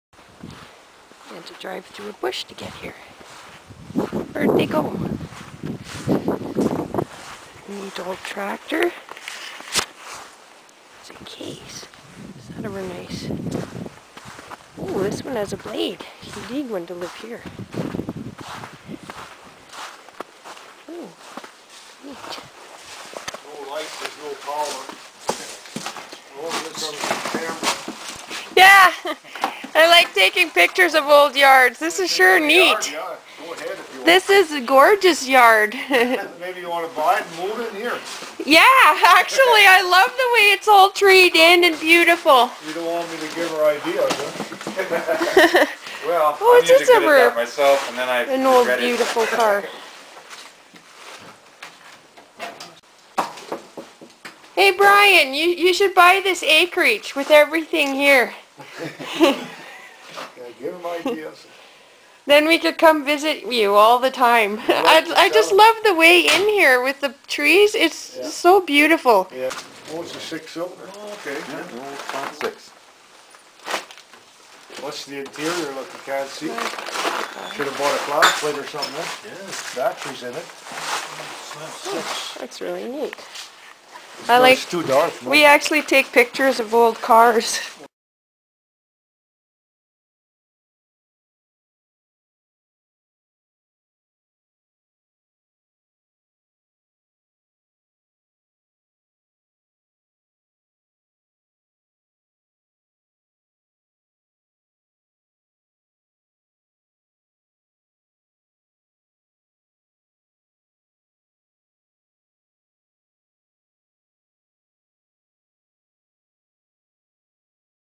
66 Fury II 4 door Barn Find
Looking at a old Plymouth 3/4 of a mile off any main road at an old farm yard.